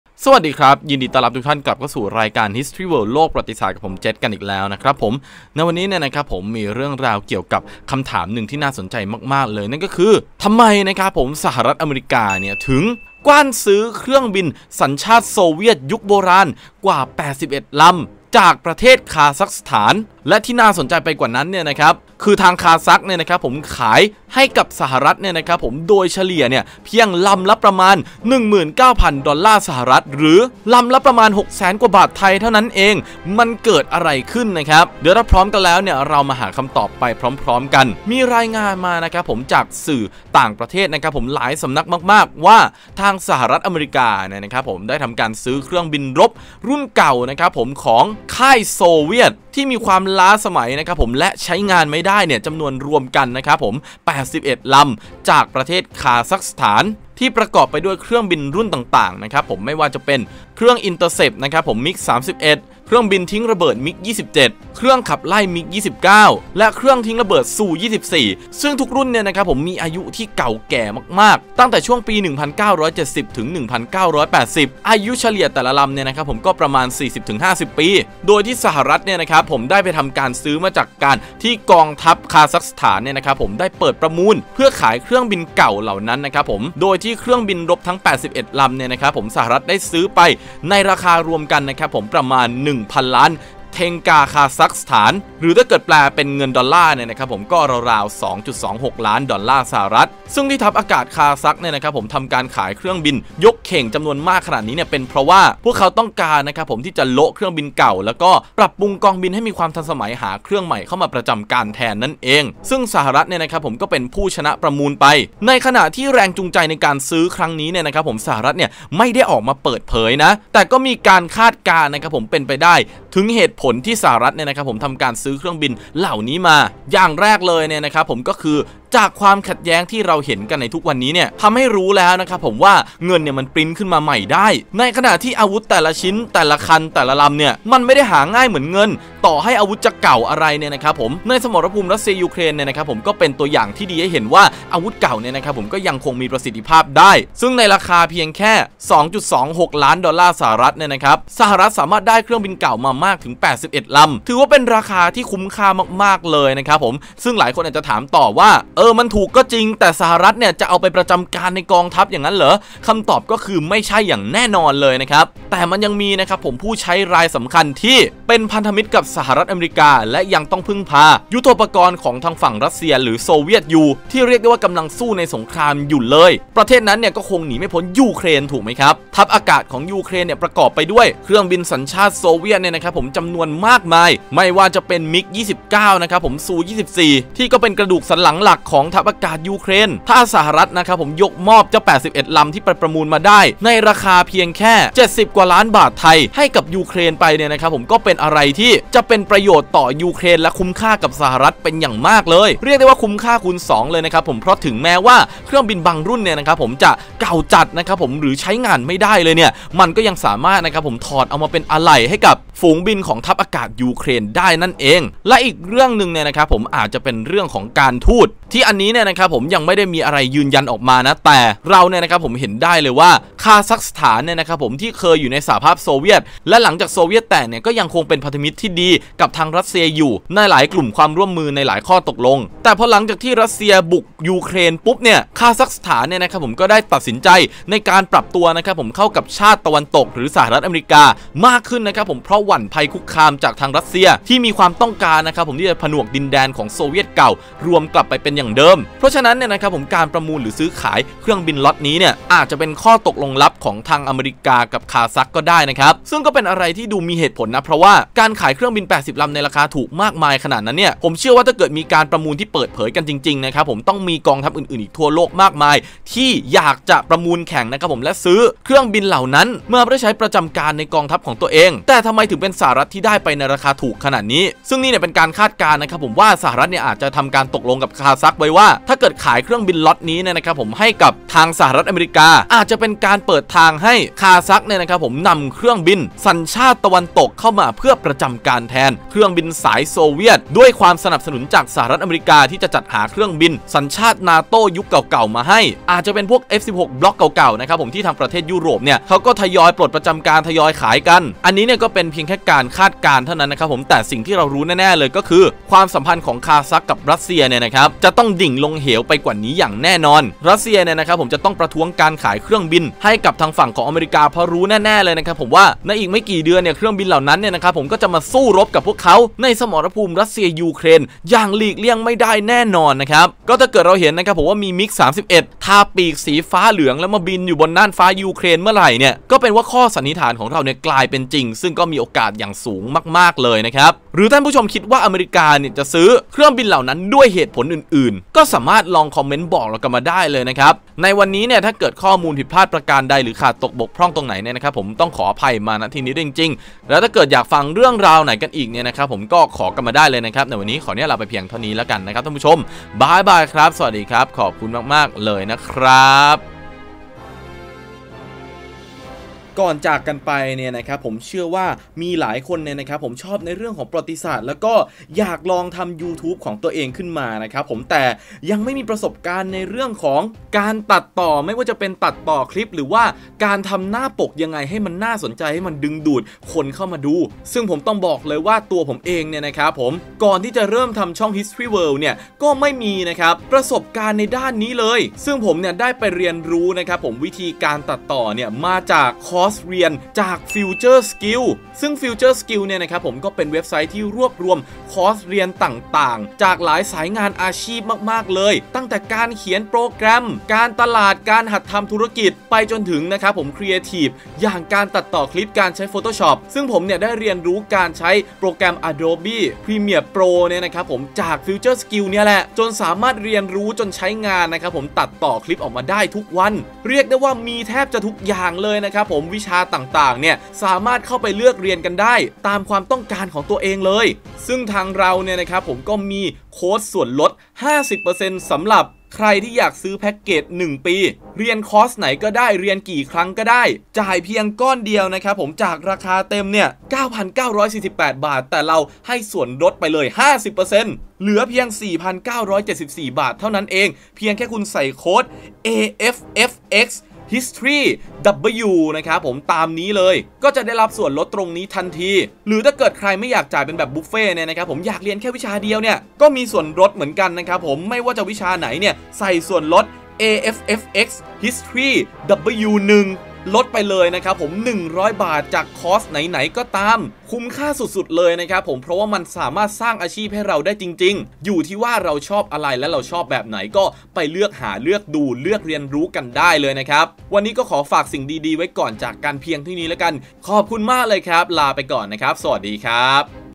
ทำไมสหรัฐเหมาซื้อ ฝูงบินโซเวียตทั้งฝูง? แถมได้ซื้อในราคาลำละแค่ 7 แสน ถูกกว่าซื้อรถ? - History World
สนใจสั่งซื้อรถถังจิ๋ว ติดต่อโฆษณา หรือ สนับสนุนเป็นสปอนเซอร์ให้รายการ สามารถติดต่อได้ที่